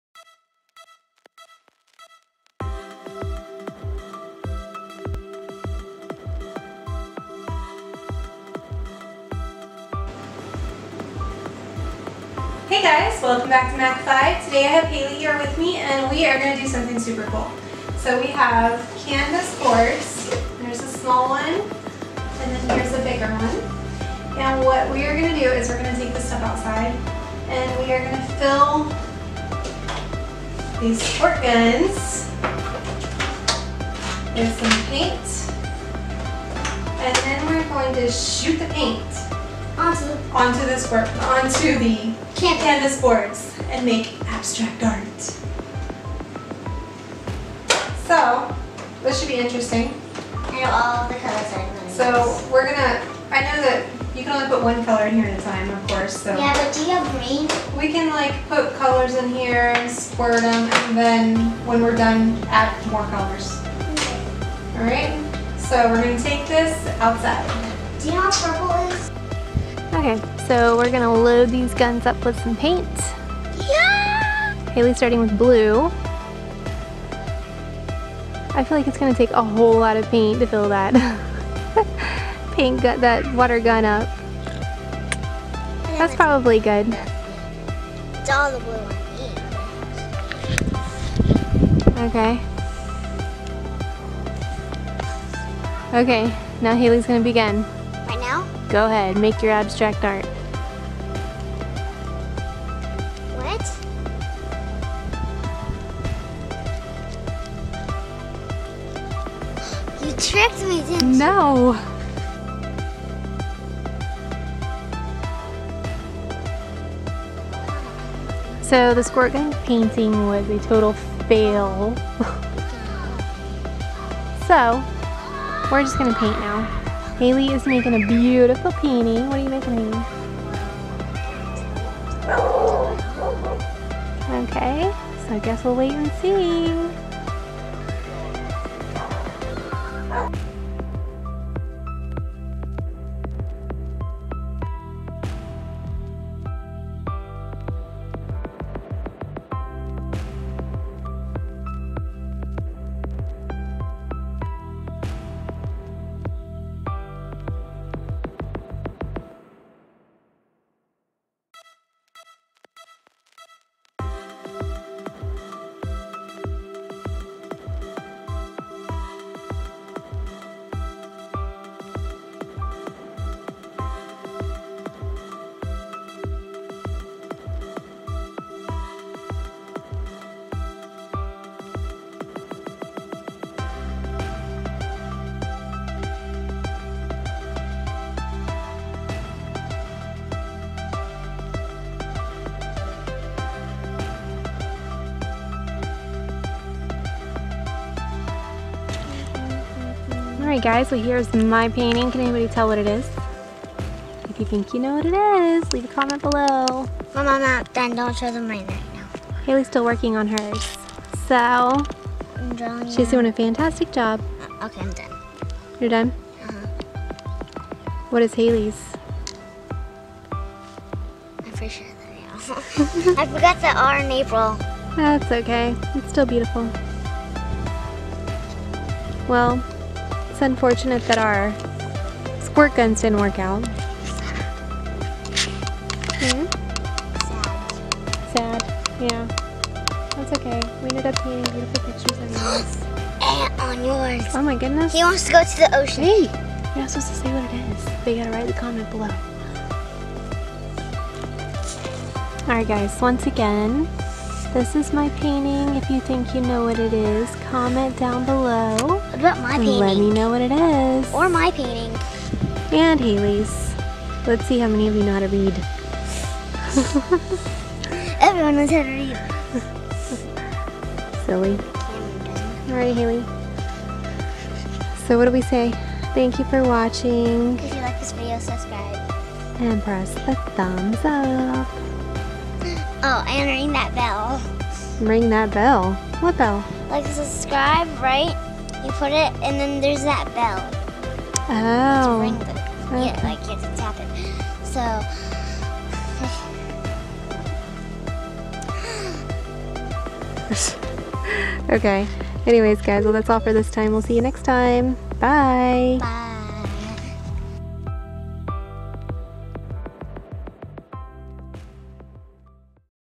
Squirt gun abstract painting FAIL! Mac 5 family! DIY. Family friendly video for kids!
Watch Hayleigh of Mac 5 attempt to paint abstract art with a squirt gun and FAIL in this fun kids video!